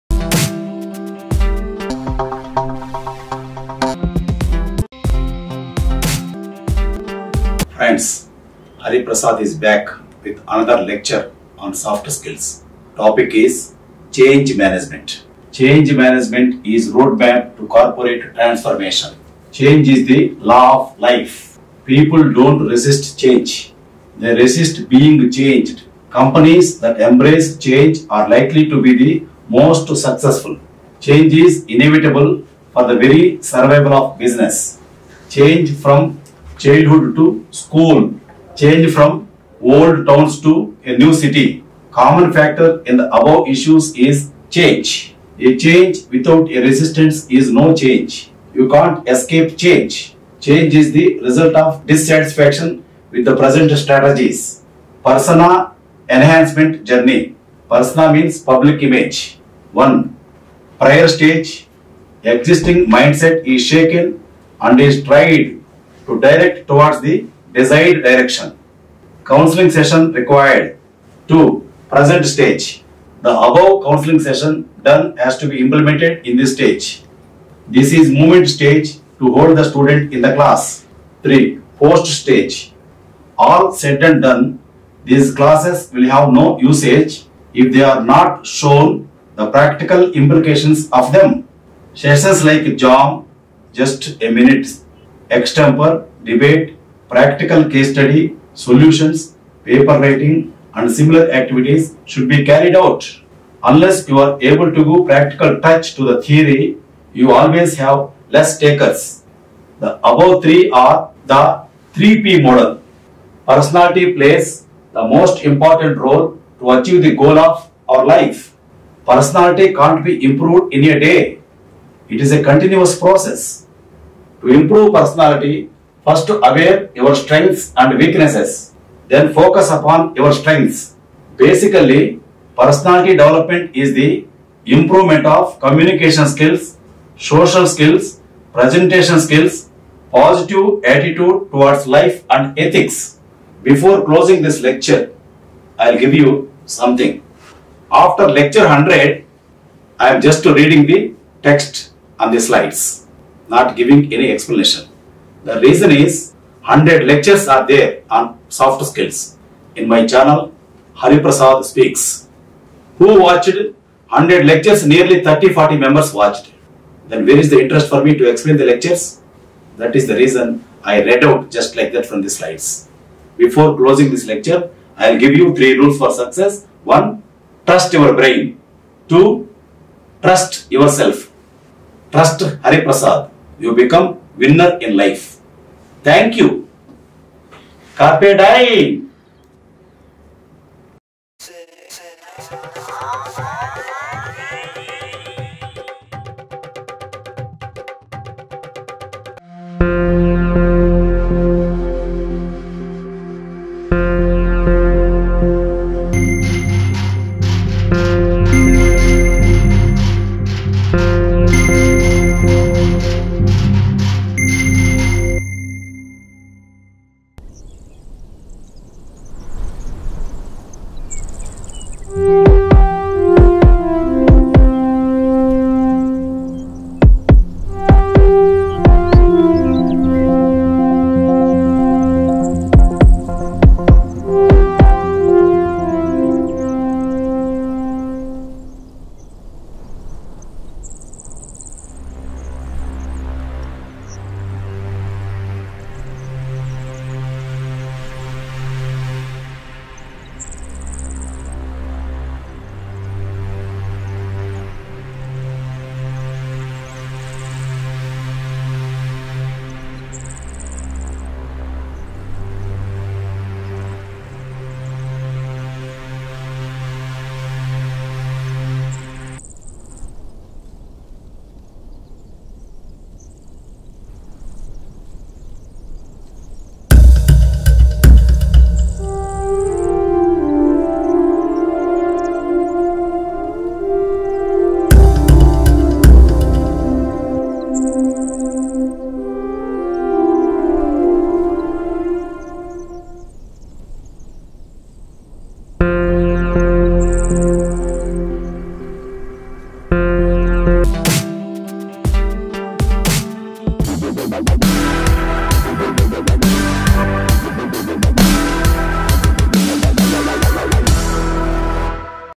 Soft Skills Episode 108 Change Management
Without change nothing happens in this world as shown in this lecture